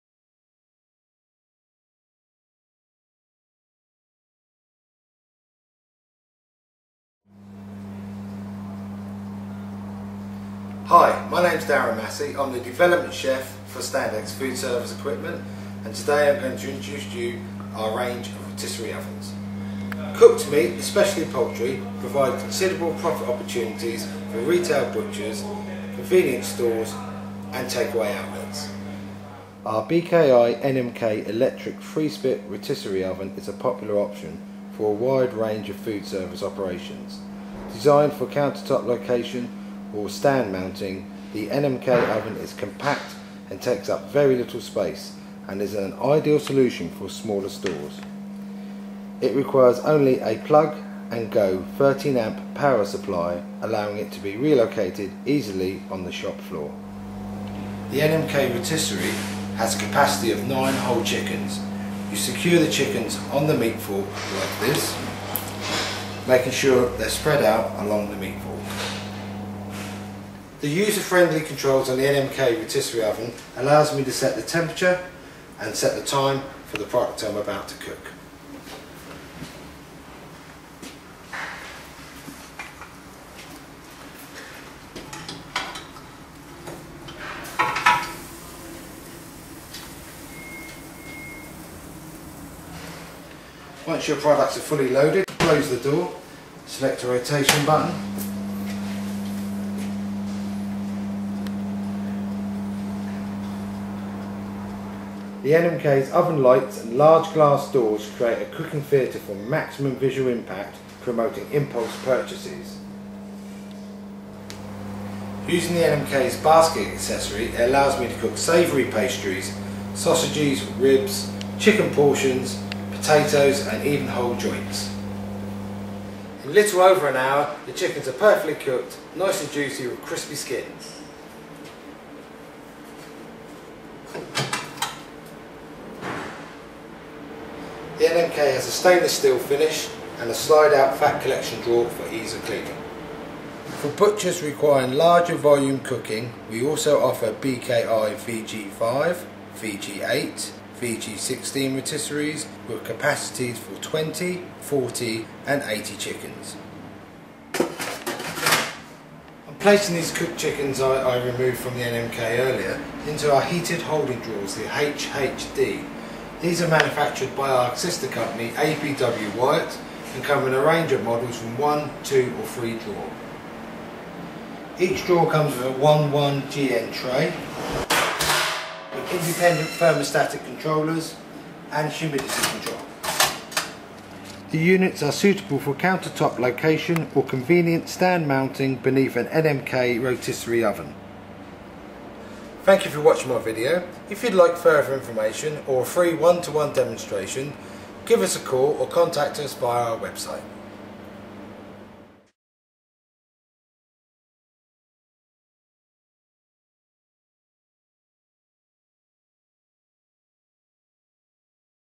Rotisserie oven video demonstration
Our Development Chef, Darren Massey, has produced a video presentation, demonstrating our range of rotisseries, placing particular emphasis on the NMK electric 3 spit model, manufactured by our sister company, BKI.